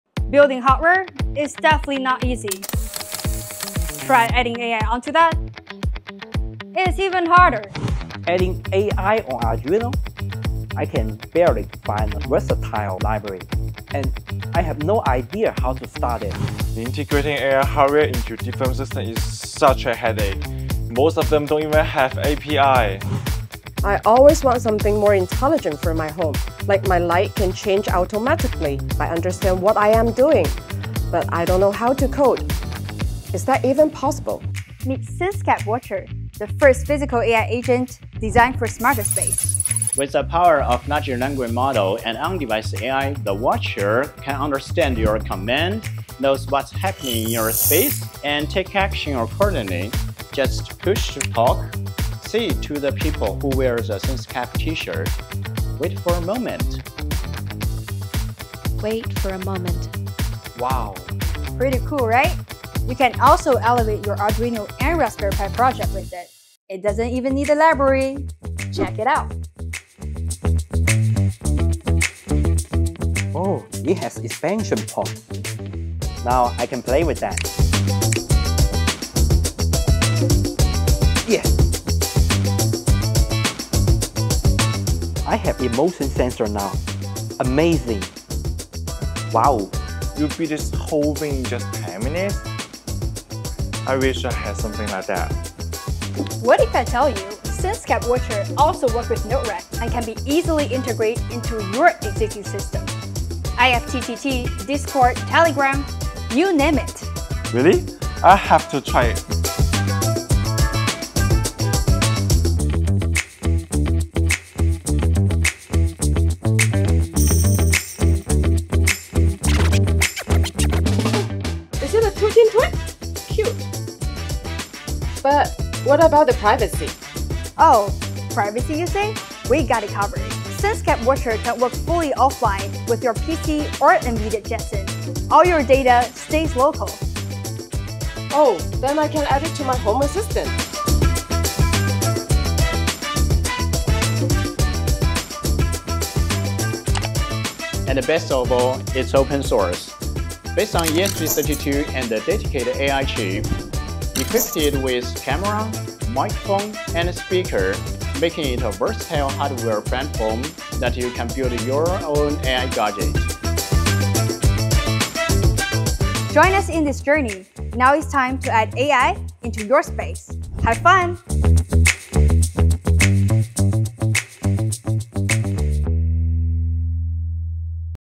SenseCAP Watcher: The Physical AI Agent for Smarter Spaces
SenseCAP Watcher is built on ESP32S3, incorporating a Himax WiseEye2 HX6538 AI chip with Arm Cortex-M55 & Ethos-U55, excelling in image and vector data processing. Equipped with a camera, microphone, speaker, SenseCAP Watcher can see, hear, talk. Plus, with LLM-enabled SenseCraft suite, SenseCAP Watcher understands your commands, perceives its surroundings, and triggers actions accordingly.